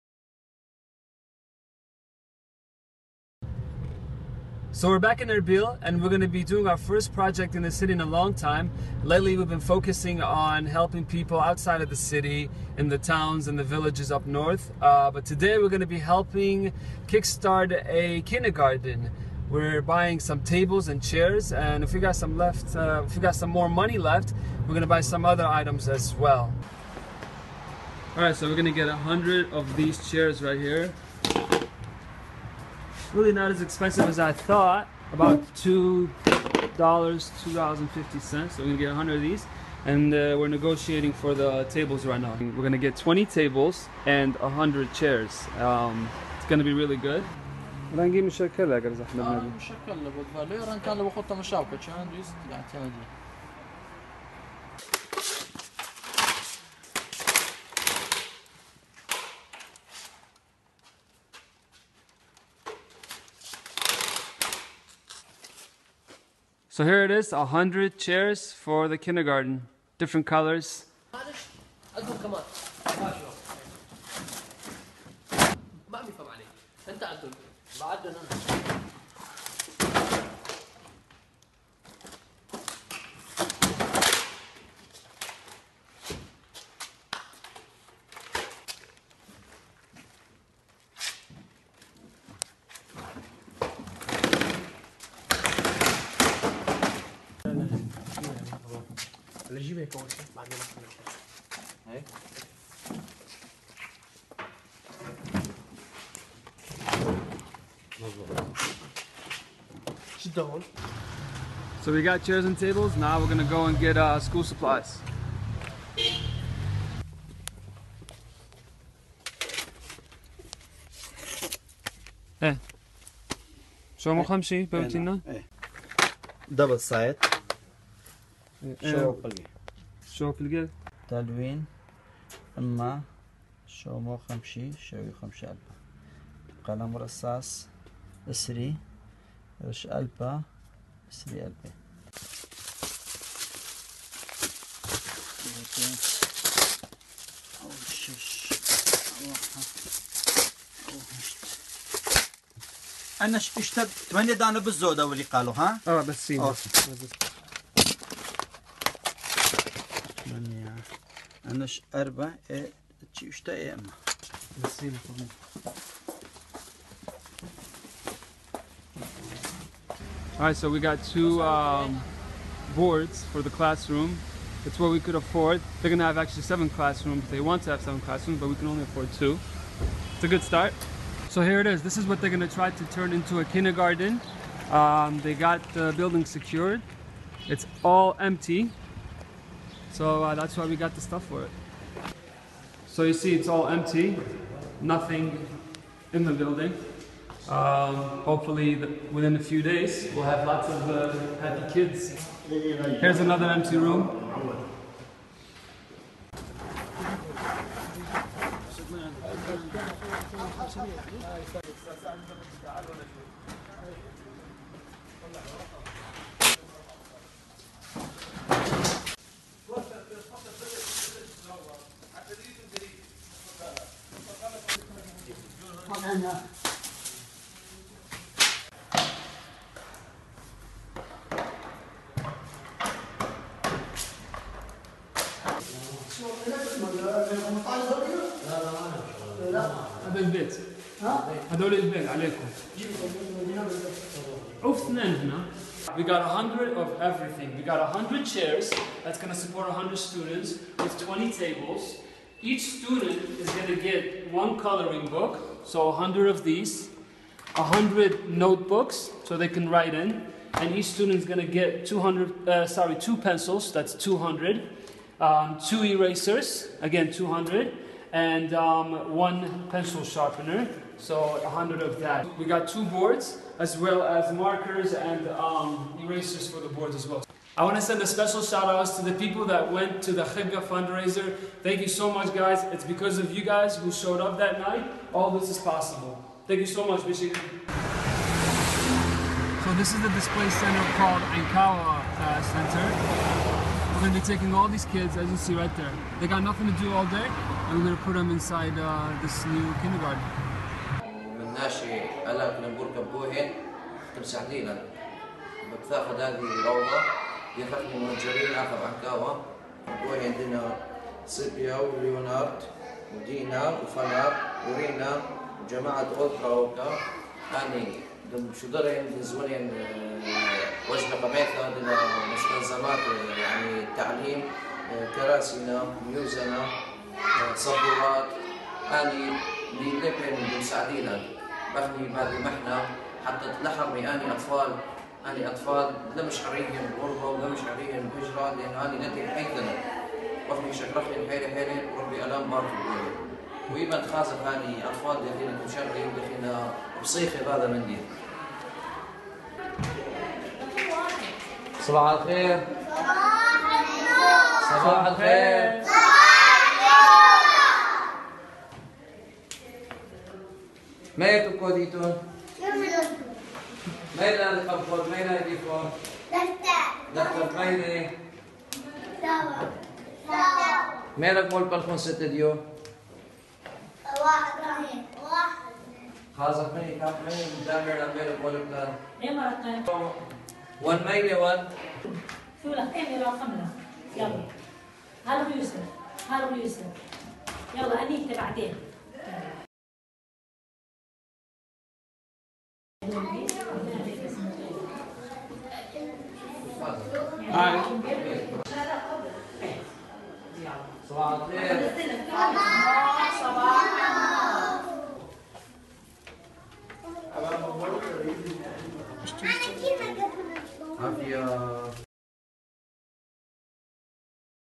2014.12.02 - Kick starting a kindergarten for displaced kids from Nineveh Plains
It's been more than 5 months since the children of Nineveh Plains have not had a normal life. Living in camps can create distress for them. The leaders recently opened a new kindergarten for displaced kids from Bartella, Behzani and other southern Nineveh Plain towns. With your donations, we were able to buy 100 chairs, 20 desks, 2 large white boards, 100 note books, 100 coloring books, 200 pencils, 100 packets of coloring pencils, 200 erasers, 100 sharpeners, and much more! Thank you!